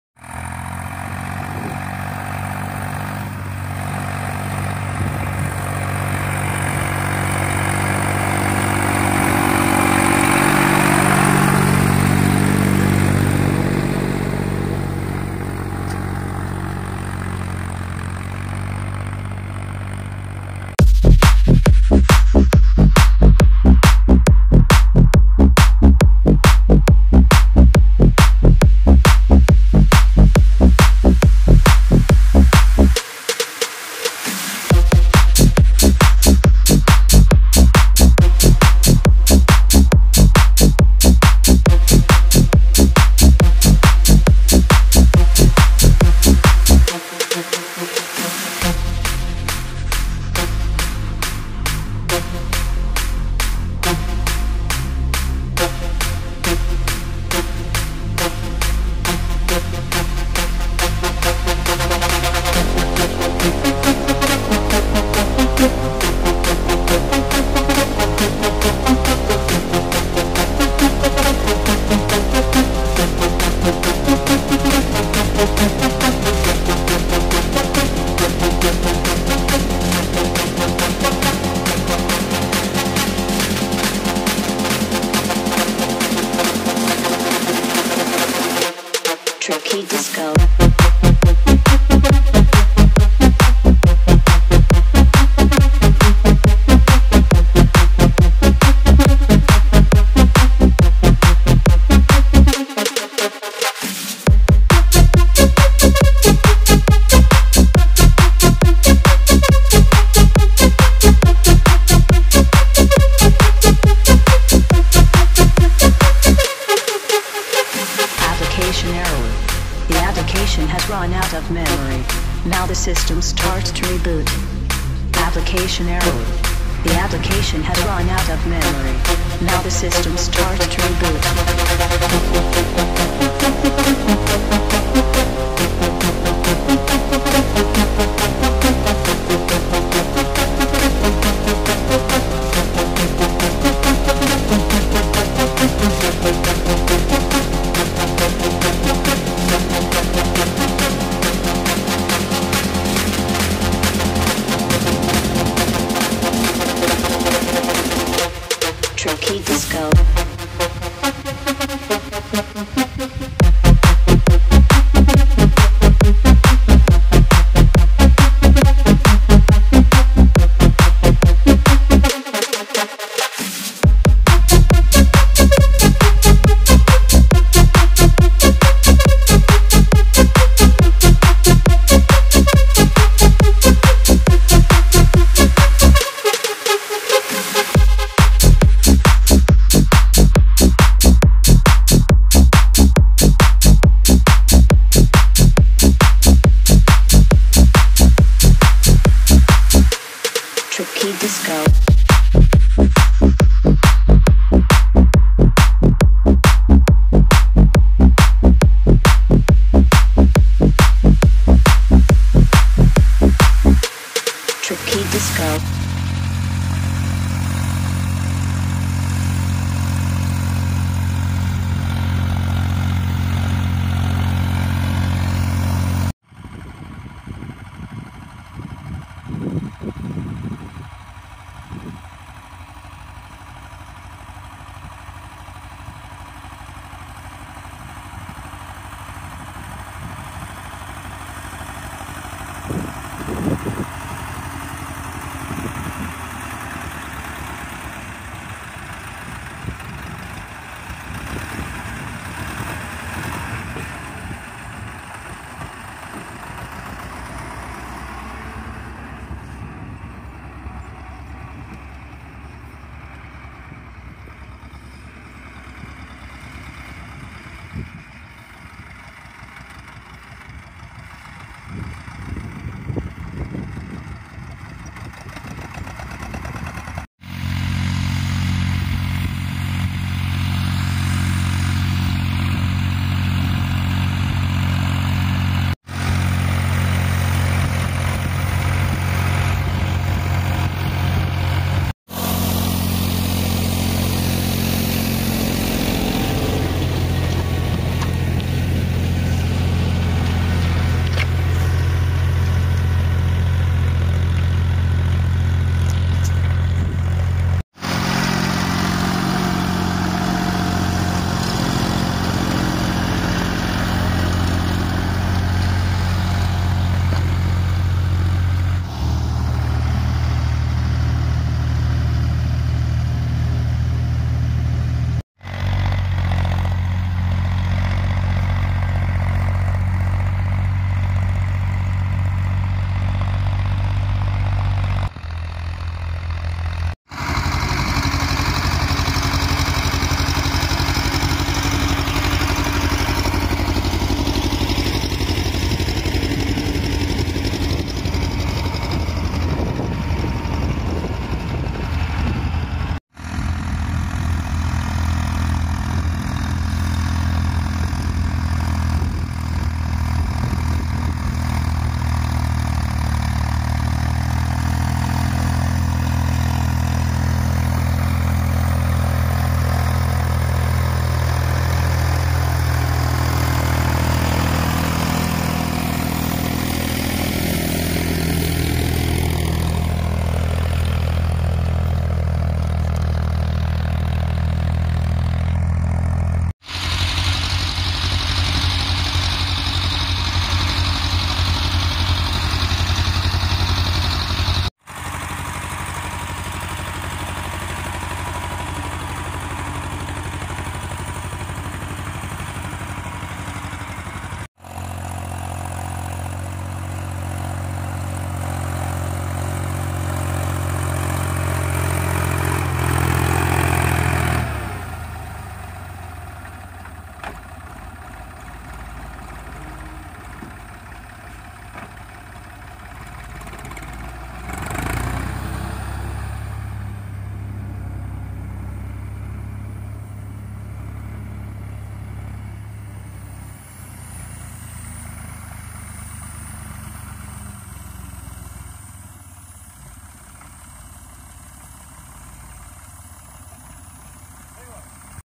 Orka C360 3p Wladimirec T25, Unia Grudziądz 6 skib ursus c360 3p 2021
Orka pod pszenżyto 11 październik 2021
W akcji :
Wladimirec T-25 A1 - kierowca : Tata
Ursus C360 3p - kierowca : Mateusz
Orka na 6 skib
Film bez muzyki od 3:40